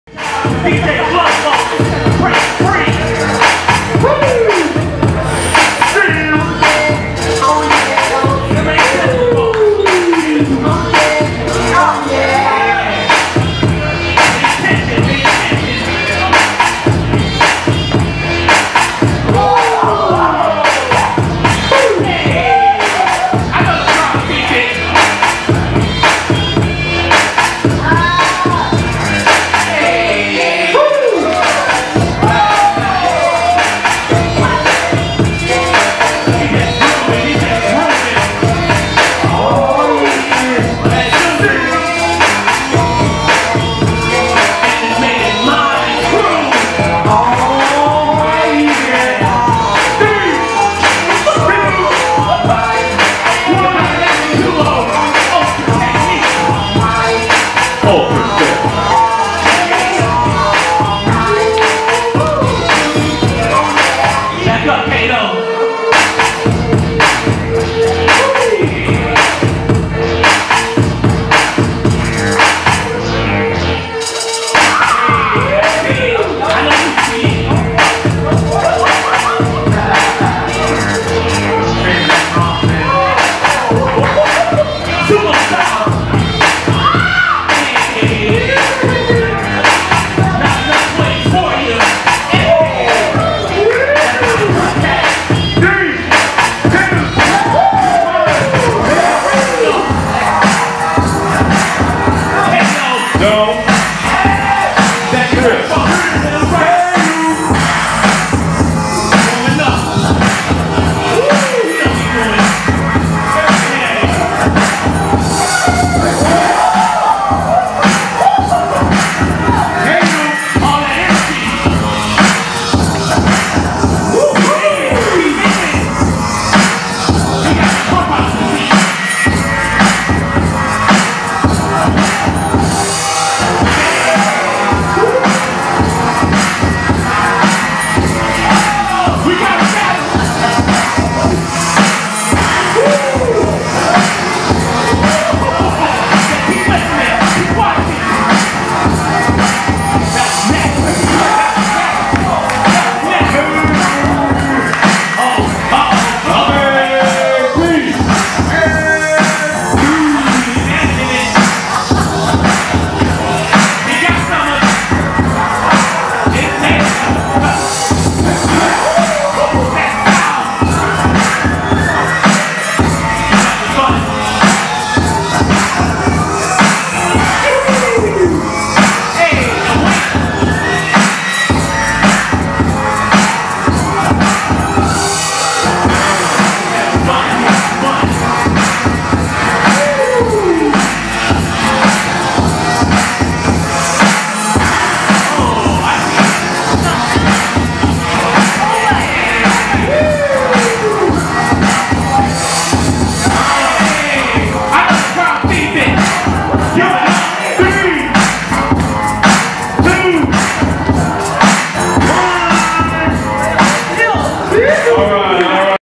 Don't Trip v (Ultratech)
YA Battles on Thursday, December 18, 2014 at The Island, St. Martin's Episcopal Church in Houston, Texas with DJ Flo